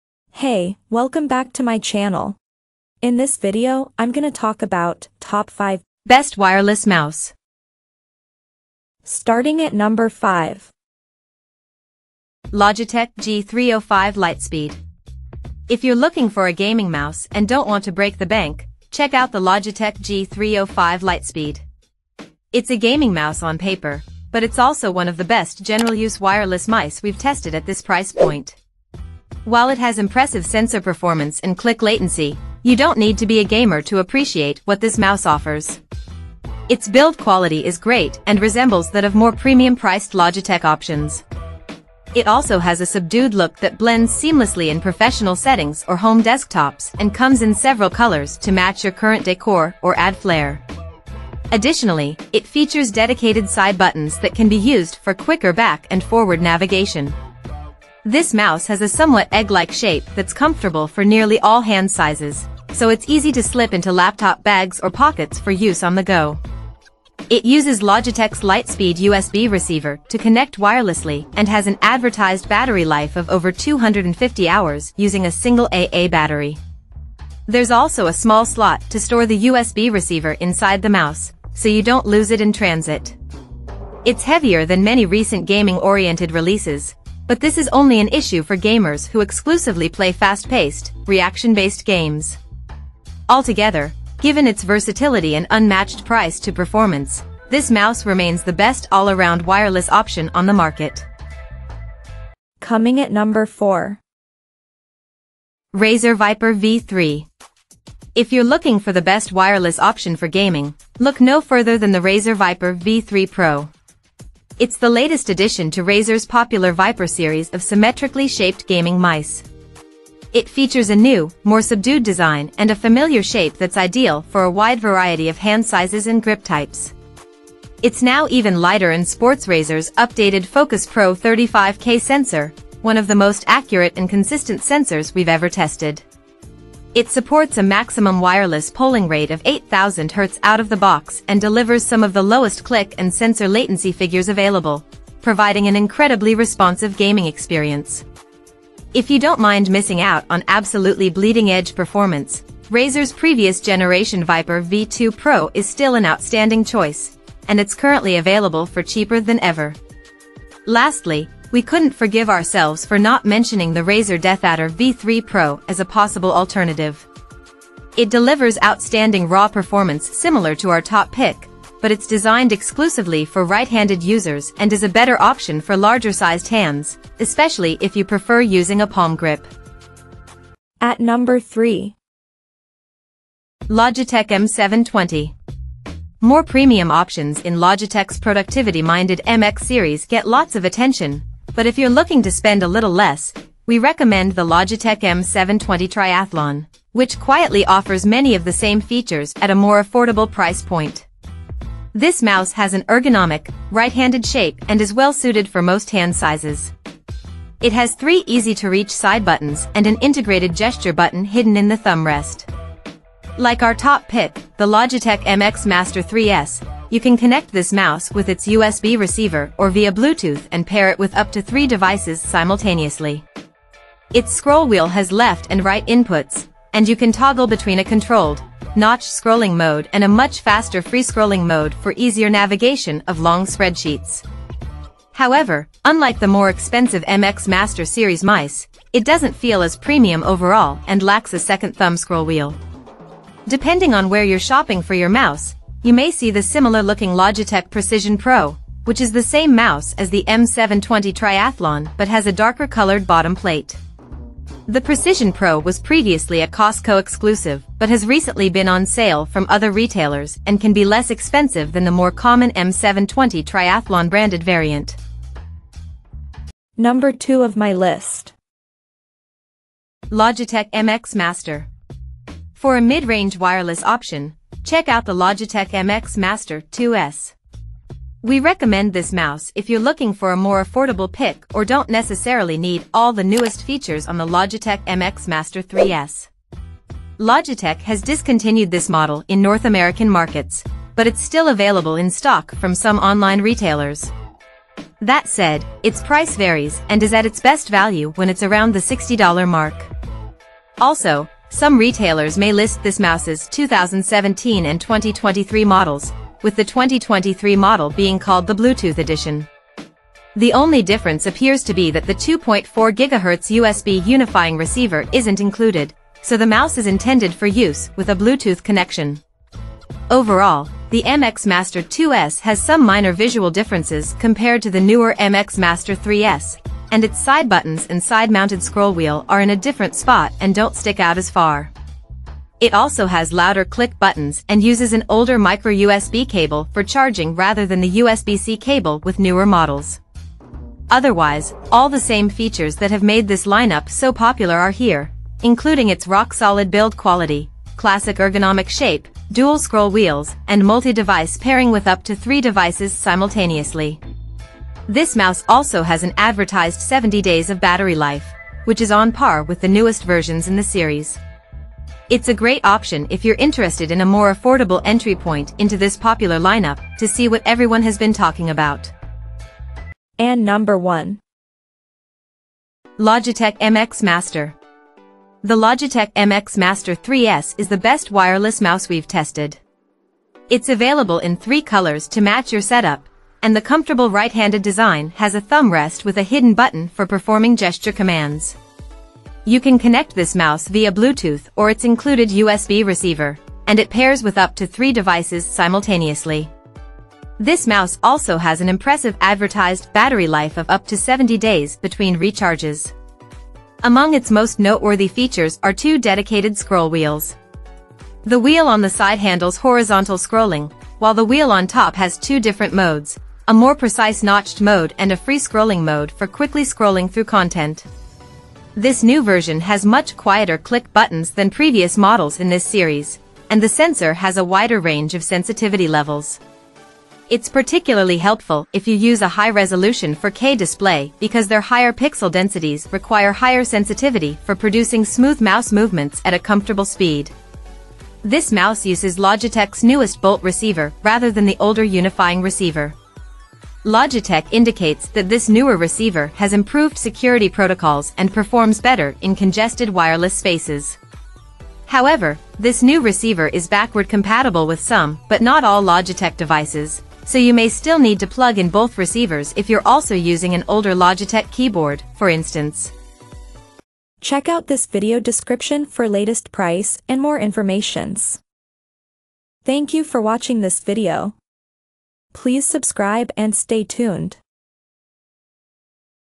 Top 5 Best Wireless Mouse in 2024
Here is the Updated Prices of the Best Wireless Mouse in 2023!

Things that we mentioned in this video:
1. Logitech MX Master  - B0BS9VVQPD
2. Logitech MX Master  - B0C4XXH2FV
3. Logitech M720 - B087Z6LSHW
4. Razer Viper V3  - B0CW25XR5S
5. Logitech G305 LIGHTSPEED - B07CMS5Q6P